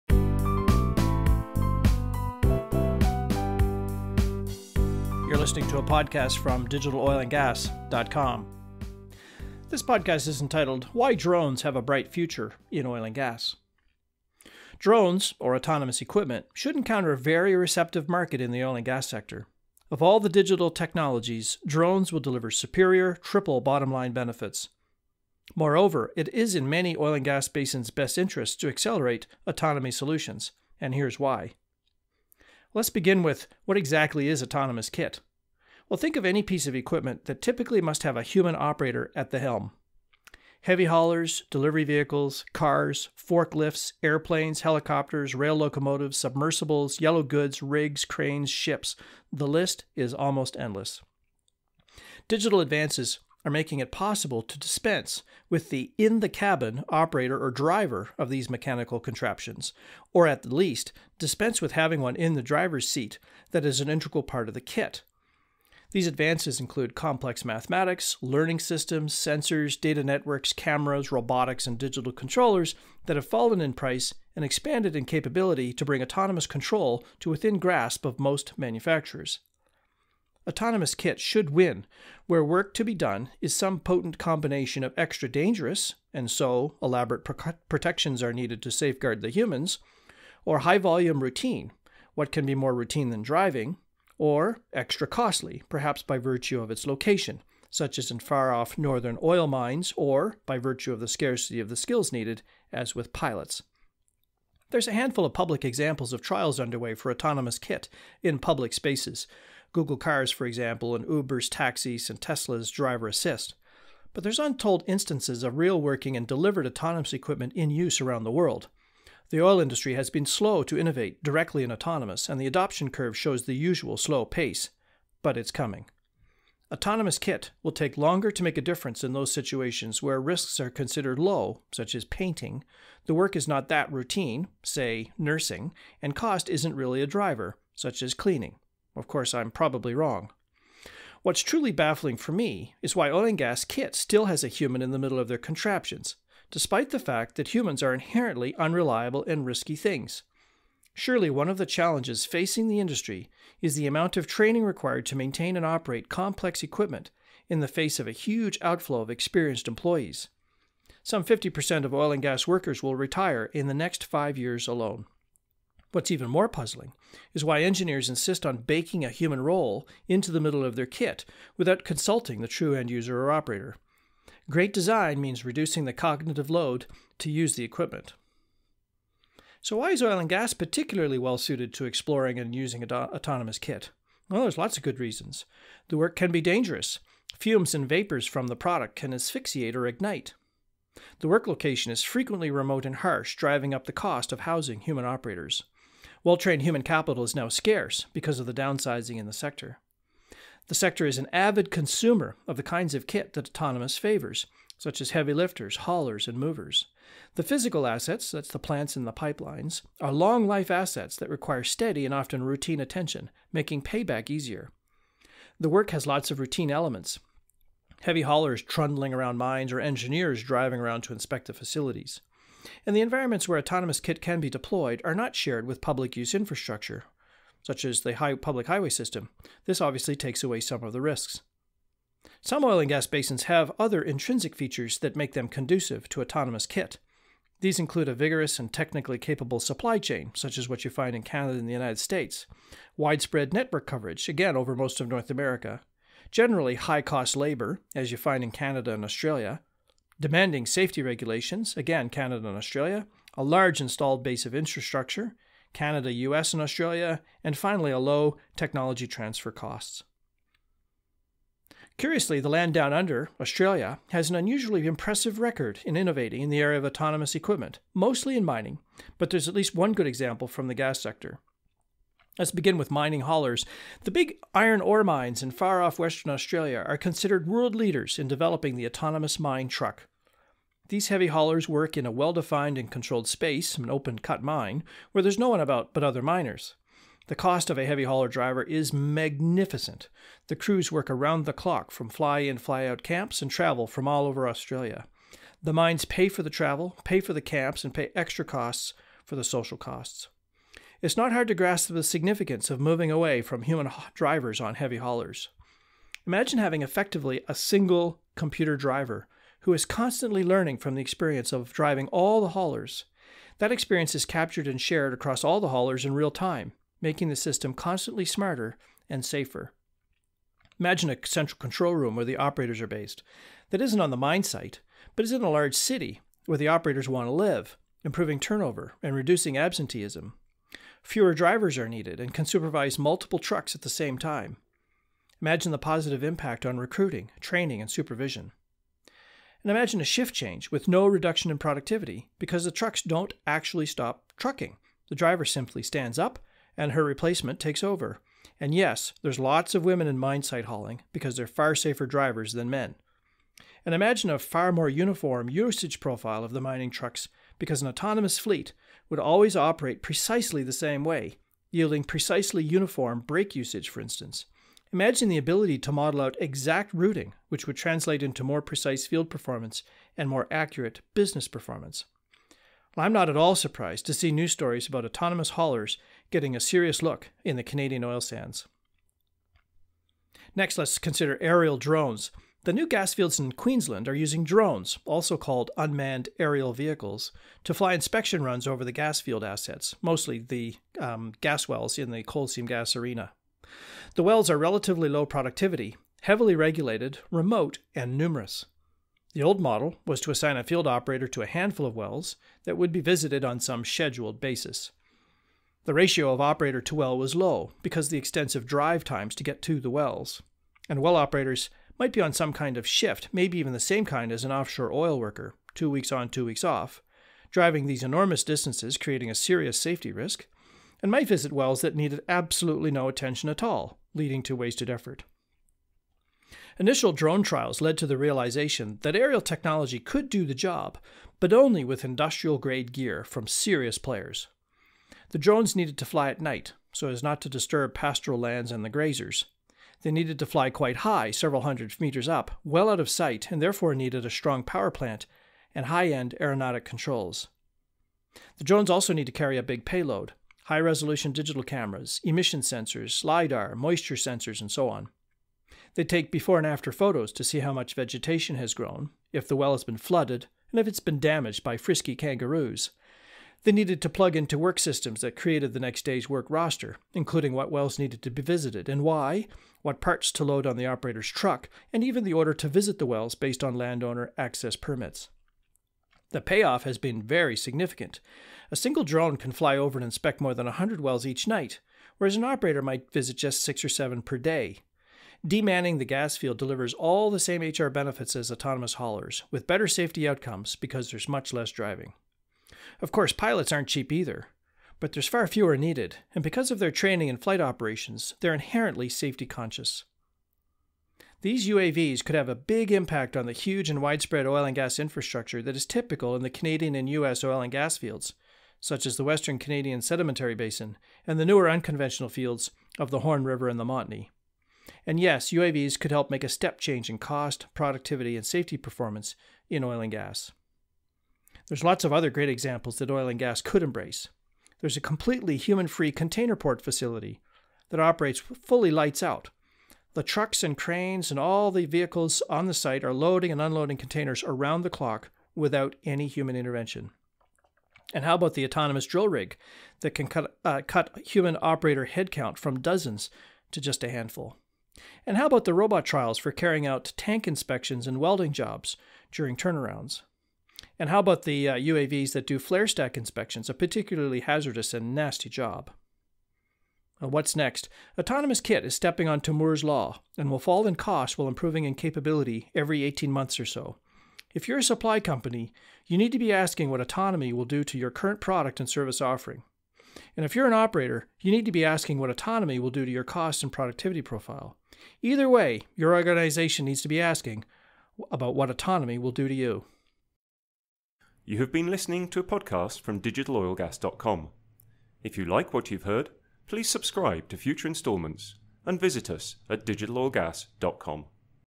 4 – WHY DRONES HAVE A BRIGHT FUTURE IN OIL AND GAS
Drones (or autonomous equipment) should encounter a very receptive market in the oil and gas sector. Of all the digital technologies, drones will deliver superior triple bottom line benefits. Moreover, it is in many oil and gas basins’ best interests to accelerate autonomy solutions.